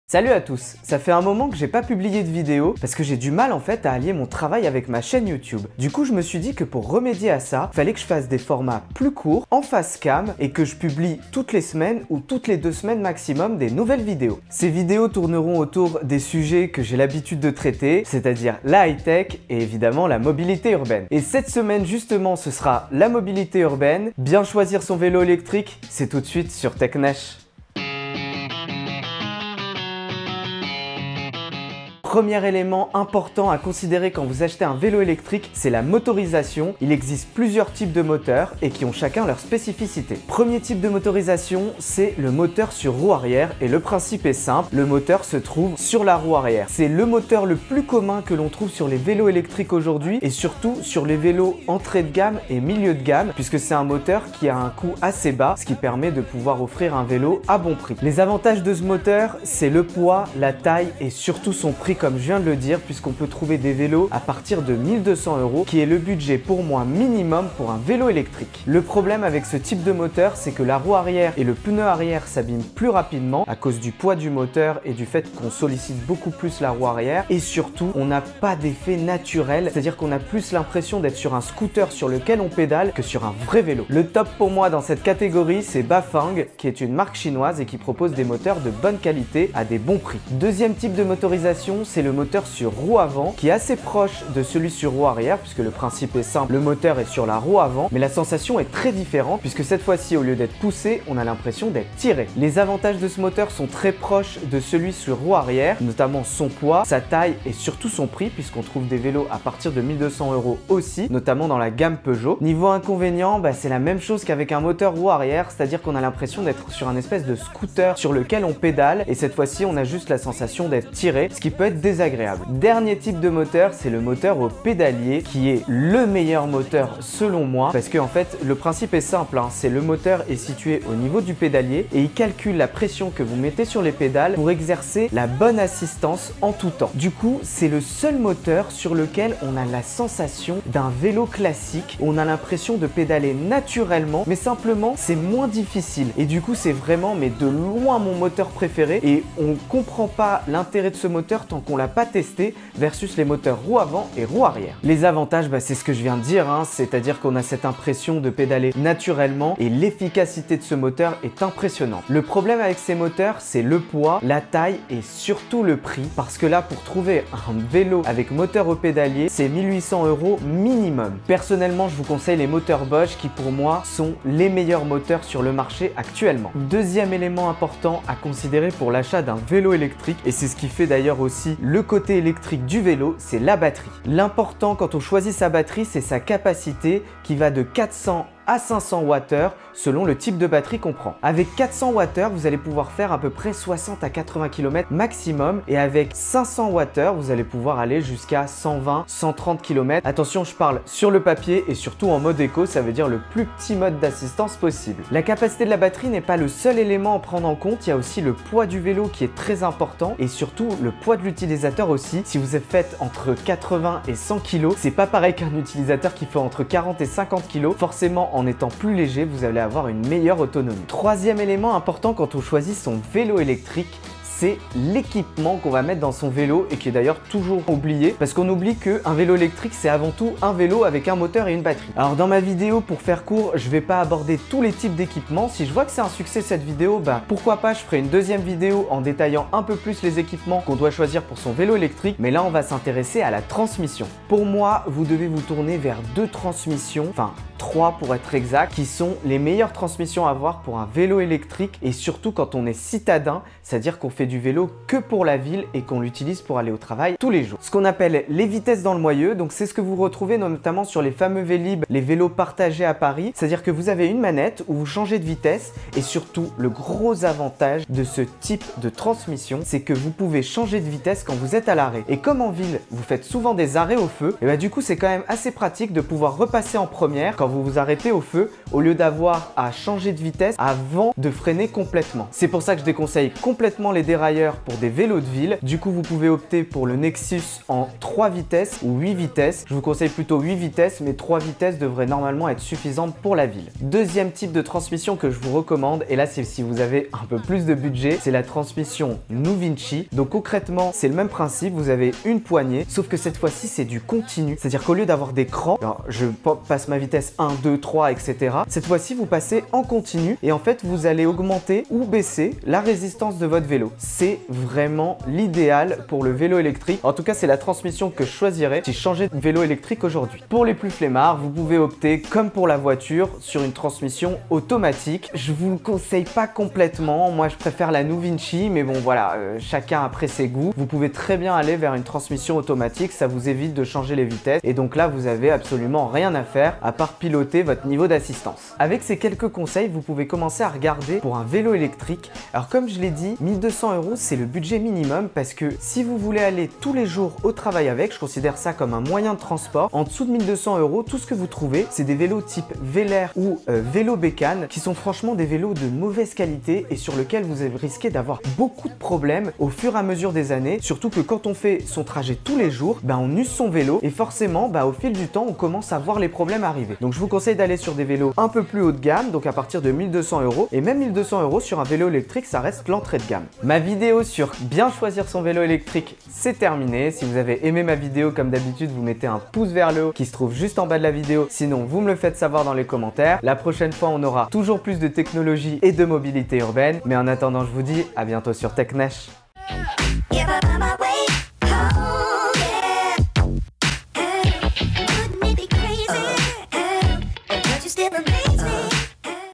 Mes conseils pour acheter un vélo électrique !
Cette semaine, je vous propose une vidéo sur comment bien choisir son vélo électrique. Je vous parle notamment des différents moteurs disponibles actuellement sur le marché, bien choisir sa batterie et sa capacité et quel type de transmission est le plus adapté pour un vélo électrique urbain.
Perso, j'ai acheté un vélo électrique sur internet (le mate bike) que je ne vous recommande pas si vous souhaitez un vélo pour vos trajets quotidiens.

Comme d'habitude, n'hésitez pas à commenter, liker, et partager la vidéo. Pour me poser des questions, c'est soit dans les commentaires de cette vidéo, soit directement via mes comptes :